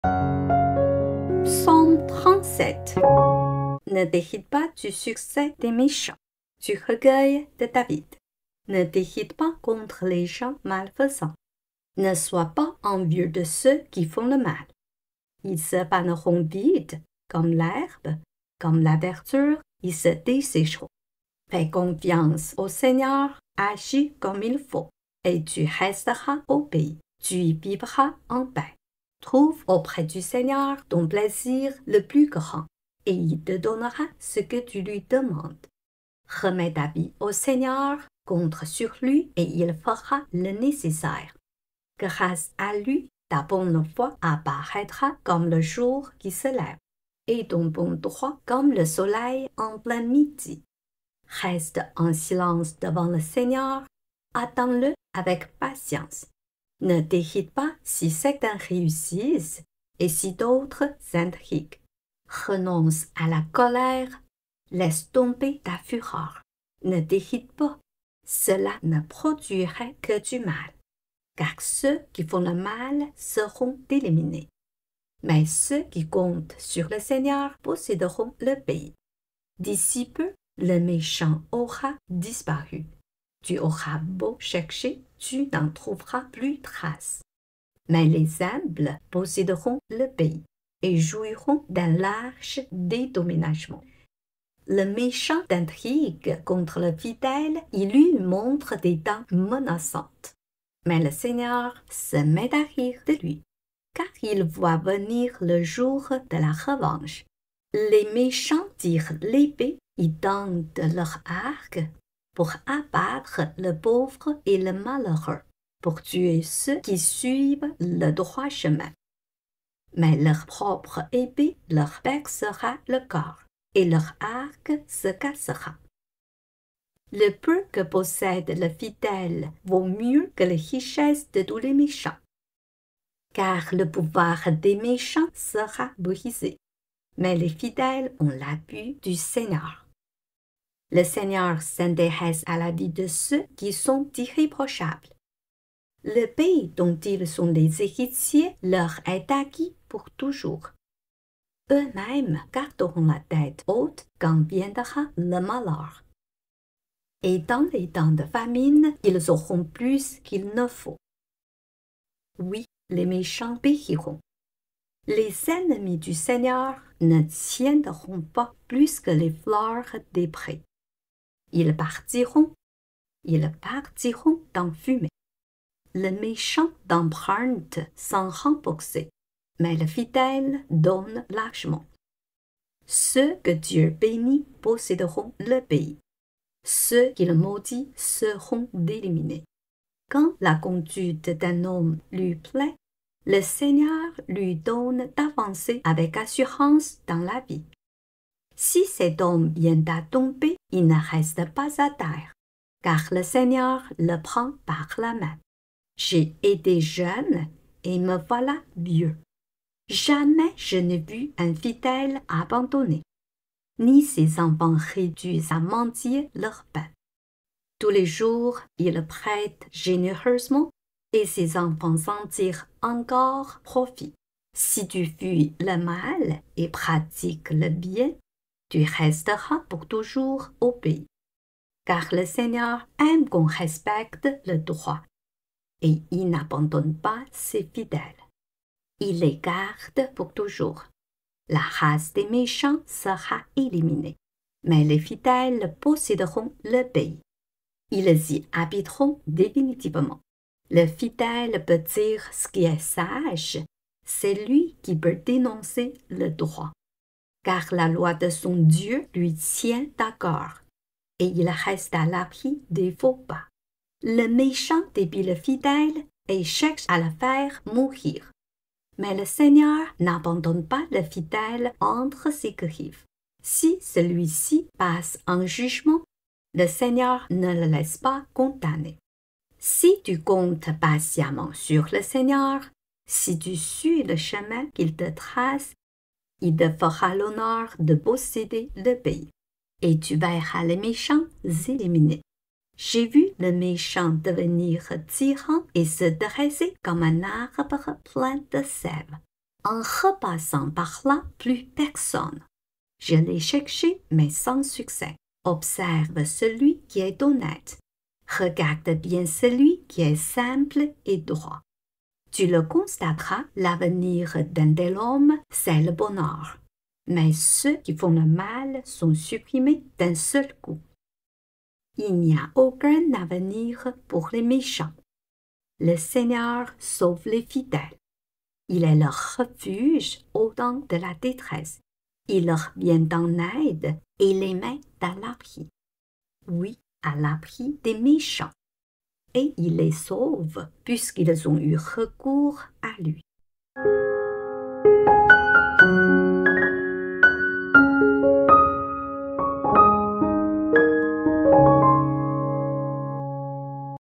Psaume 37 : Le sort du juste et de l'impie : Prière dans l'épreuve
Psaumes  37  Ne t'irrite pas du succès des méchants

1Ne t’irrite pas contre les gens malfaisants, ne sois pas envieux de ceux qui font le mal :
2ils se faneront vite, comme l’herbe, comme la verdure ils se dessécheront.
3Fais confiance au Seigneur, agis comme il faut, et tu resteras au pays, tu y vivras en paix ;
4Trouve auprès du Seigneur ton plaisir le plus grand, et il te donnera ce que tu lui demandes.
5Remets ta vie au Seigneur, compte sur lui, et il fera le nécessaire.
6Grâce à lui ta bonne foi apparaîtra comme le jour qui se lève, et ton bon droit comme le soleil en plein midi.
7Reste en silence devant le Seigneur, attends-le avec patience. Ne t’irrite pas si certains réussissent et si d’autres intriguent.
8Renonce à la colère, laisse tomber ta fureur. Ne t’irrite pas, cela ne produirait que du mal.
9Car ceux qui font le mal seront éliminés, mais ceux qui comptent sur le Seigneur posséderont le pays.
10D’ici peu, le méchant aura disparu ; tu auras beau chercher, tu n’en trouveras plus trace.
11Mais les humbles posséderont le pays et jouiront d’un large dédommagement.
12Le méchant intrigue contre le fidèle, il lui montre des dents menaçantes.
13Mais le Seigneur se met à rire de lui, car il voit venir le jour de la revanche.
14Les méchants tirent l’épée, ils tendent leur arc pour abattre le pauvre et le malheureux, pour tuer ceux qui suivent le droit chemin.
15Mais leur propre épée leur percera le cœur et leur arc se cassera.
16Le peu que possède le fidèle vaut mieux que les richesses de tous les méchants,
17car le pouvoir des méchants sera brisé, mais les fidèles ont l’appui du Seigneur.
18Le Seigneur s’intéresse à la vie de ceux qui sont irréprochables. Le pays dont ils sont les héritiers leur est acquis pour toujours.
19Eux-mêmes garderont la tête haute quand viendra le malheur, et dans les temps de famine ils auront plus qu’il ne faut.
20Oui, les méchants périront, les ennemis du Seigneur ne tiendront pas plus que les fleurs des prés ; ils partiront, ils partiront en fumée.
21Le méchant emprunte sans rembourser, mais le fidèle donne largement.
22Ceux que Dieu bénit posséderont le pays, ceux qu’il maudit seront éliminés.
23Quand la conduite d’un homme lui plaît, le Seigneur lui donne d’avancer avec assurance dans la vie.
24Si cet homme vient à tomber, il ne reste pas à terre, car le Seigneur le prend par la main.
25J’ai été jeune et me voilà vieux ; jamais je n’ai vu un fidèle abandonné, ni ses enfants réduits à mendier leur pain.
26Tous les jours il prête généreusement et ses enfants en tirent encore profit.
27Si tu fuis le mal et pratiques le bien, tu resteras pour toujours au pays,
28car le Seigneur aime qu’on respecte le droit et il n’abandonne pas ses fidèles. Il les garde pour toujours, la race des méchants sera éliminée.
29Mais les fidèles posséderont le pays, ils y habiteront définitivement.
30Le fidèle peut dire ce qui est sage, c’est lui qui peut énoncer le droit,
31car la loi de son Dieu lui tient à cœur, et il reste à l’abri des faux pas.
32Le méchant épie le fidèle et cherche à le faire mourir.
33Mais le Seigneur n’abandonne pas le fidèle entre ses griffes. Si celui-ci passe en jugement, le Seigneur ne le laisse pas condamner.
34Si tu comptes patiemment sur le Seigneur, si tu suis le chemin qu’il te trace, il te fera l’honneur de posséder le pays, et tu verras les méchants éliminés.
35J’ai vu le méchant devenir tyran et se dresser comme un arbre plein de sève.
36En repassant par là : plus personne ; je l’ai cherché, mais sans succès.
37Observe celui qui est honnête, regarde bien celui qui est simple et droit. Tu le constateras : l’avenir d’un tel homme, c’est le bonheur.
38Mais ceux qui font le mal sont supprimés d’un seul coup ; il n’y a aucun avenir pour les méchants.
39Le Seigneur sauve les fidèles, il est leur refuge au temps de la détresse.
40Il leur vient en aide et les met à l’abri, oui, à l’abri des méchants, et il les sauve, puisqu’ils ont eu recours à lui.